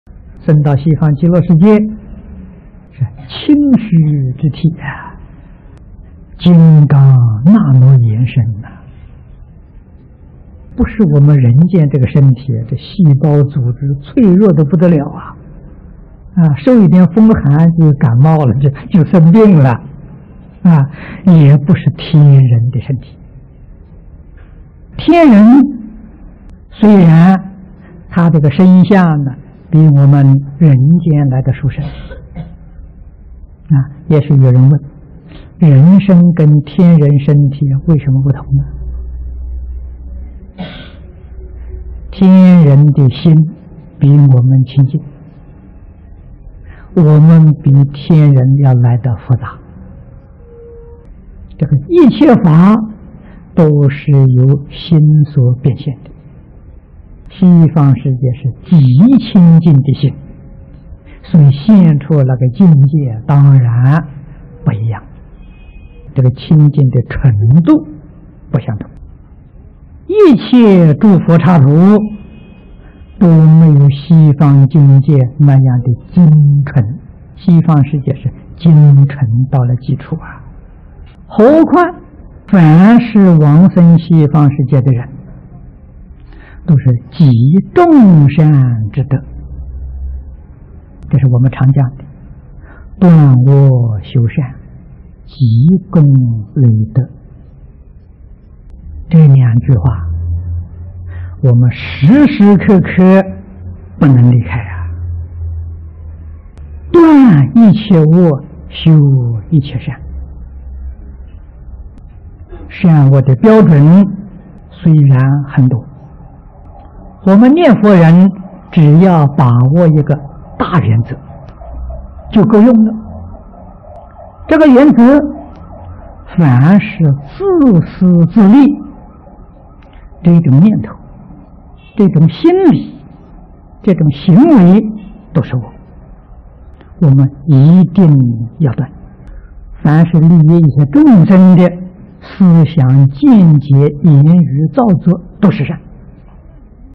淨空老法師｜念佛成佛｜西方世界精純到極處
生到西方極樂世界是清虛之體、金剛那羅延身；不是我們人間這個身體，這細胞組織脆弱得不得了，受一點風寒就感冒生病；也不是天人的身體，雖然天人的身相比我們人間殊勝。
也許有人問：人身跟天人身體為什麼不同？天人的心比我們清淨，我們比天人來得複雜。一切法都是由心所變現的，西方世界是極清淨的心，現出那個境界當然不一樣，清淨的純度不相同。
一切諸佛剎土都沒有西方境界那樣精純，西方世界是精純到極處！何況凡是往生西方世界的人，都是『積眾善之德』，這是我們常講的「斷惡修善，積功累德」，這兩句話我們時時刻刻不能離開，斷一切惡、修一切善。
善惡的標準雖然很多，我們念佛人只要把握一個大原則就夠用了。這個原則，凡是自私自利這種念頭、這種心理、這種行為都是惡，我們一定要斷；凡是利益一切眾生的思想見解、言語造作都是善。
1984/12